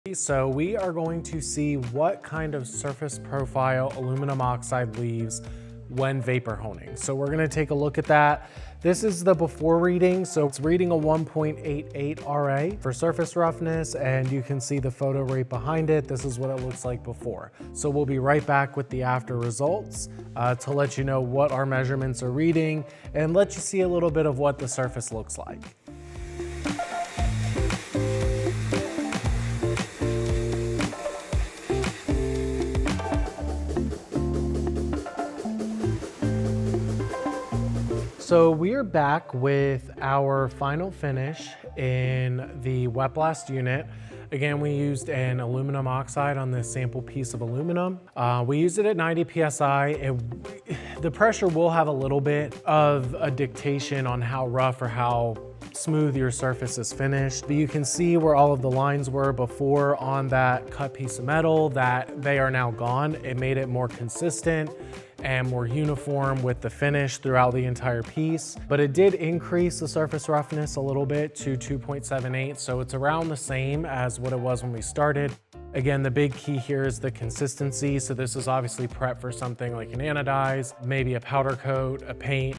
Wet Blasting with Aluminum Oxide
Have you wondered what surface profile aluminum oxide can create with vapor blasting? Today, we're putting it to the test! We have an aluminum metal piece, and we'll measure its surface roughness before and after vapor blasting to see how much of a difference it makes.